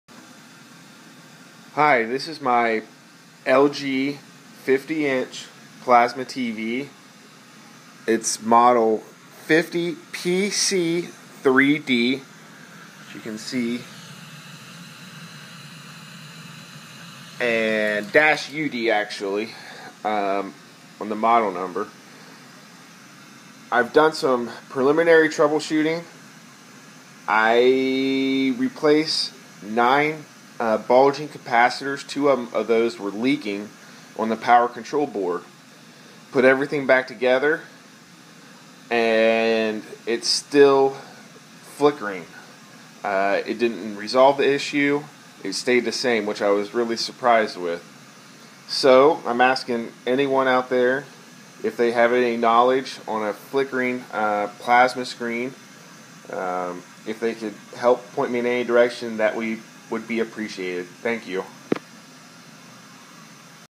Plasma TV screen flashing/flickering HELP
LG 50" Plasma TV model 50PC3D-UD. Screen flashing/flickering. I replaced 9 capacitors on the power control board thinking it would resolve the issue. I'm on the fence if I want to spend the money on a repair guy because of the age of the TV. So if I can repair myself I will try. If not, I think it may be going to an electronic recycling graveyard. Any help would be appreciated.  Matt